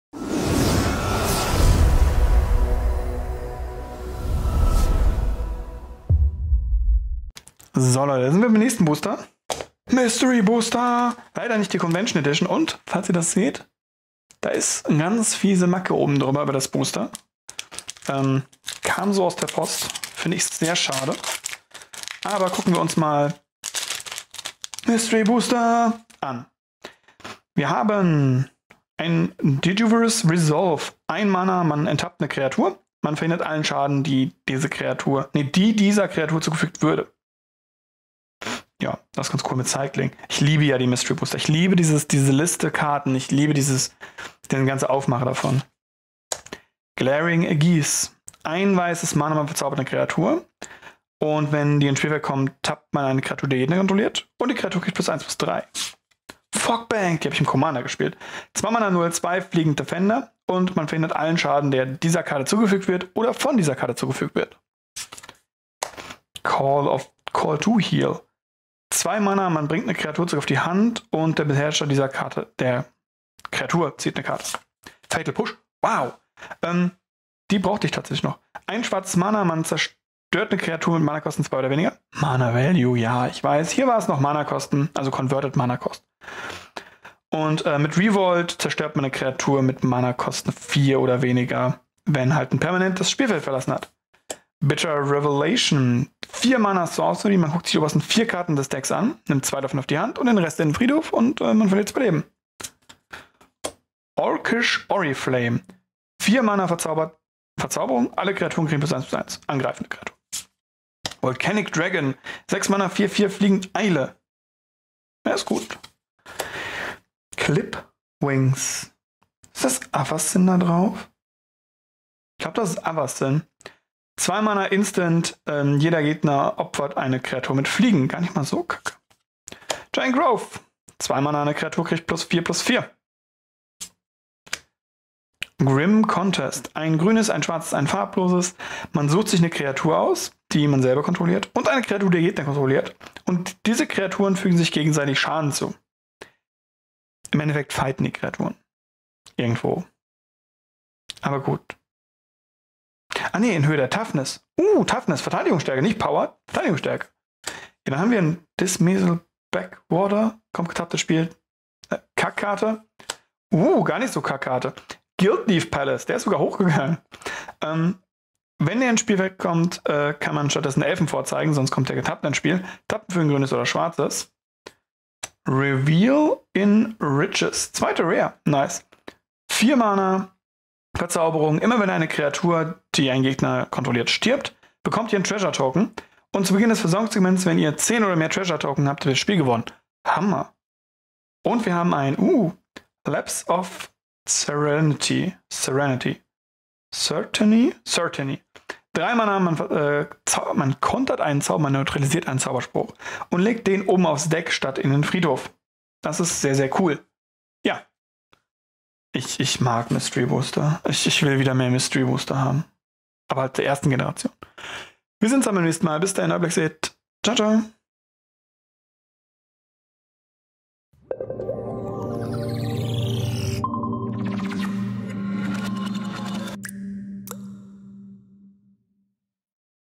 Mystery Booster Booster361
Hier war das dritte Booster von meinem "Draftkit" was ich im Laufe der Zeit aufgemacht habe.
Ich habe leider Niemanden gefunden zum Draften.